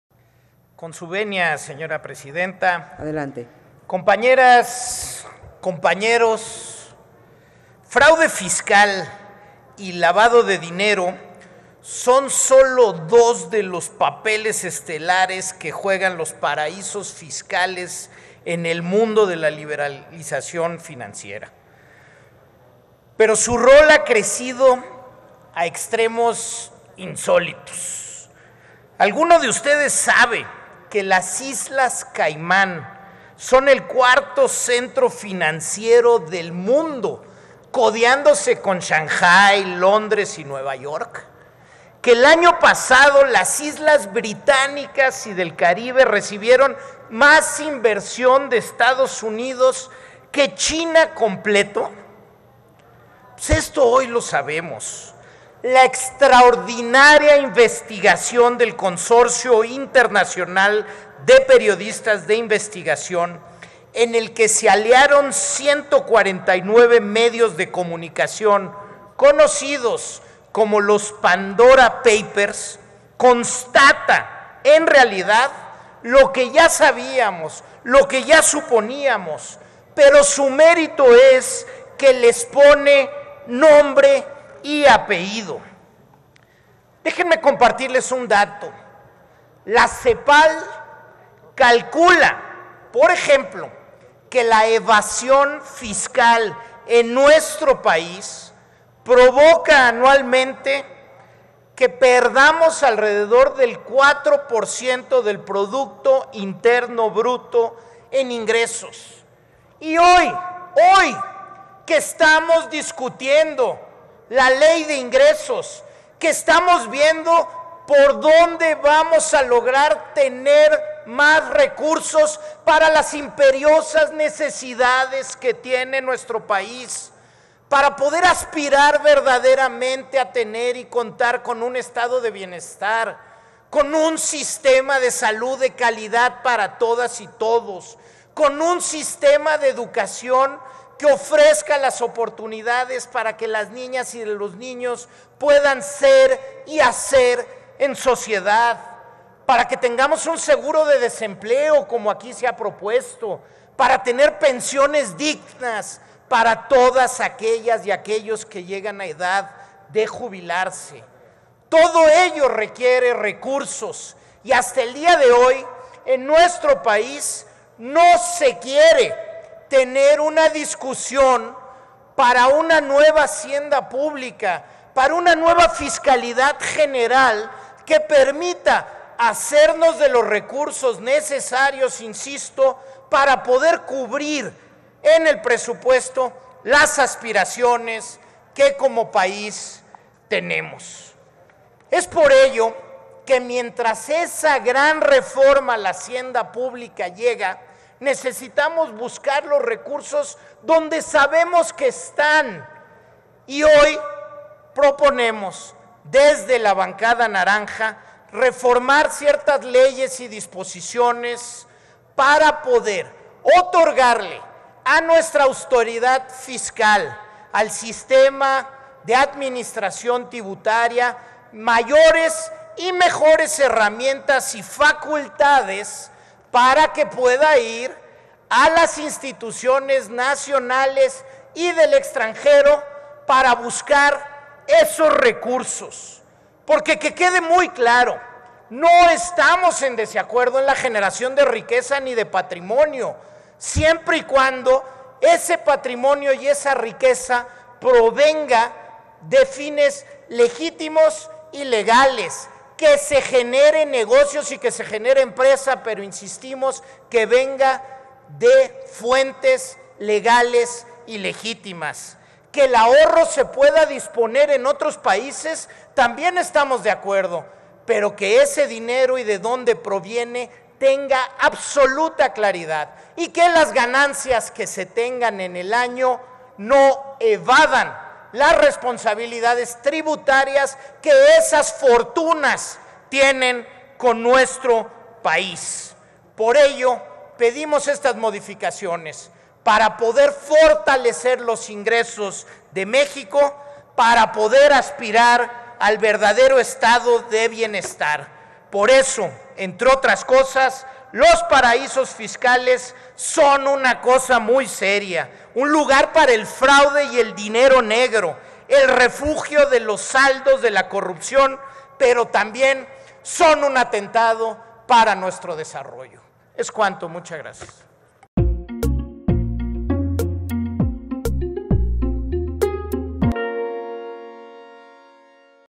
Dip. Salomon Chertorivski Woldenberg (MC) / Ley General de Responsabilidades Administrativas
El diputado Salomon Chertorivski Woldenberg (MC) presenta una iniciativa que reforma diversas leyes y códigos, a fin de evitar la evasión del pago de impuestos mediante la inversión en empresas offshore constituidas en los llamados “paraísos fiscales” en el extranjero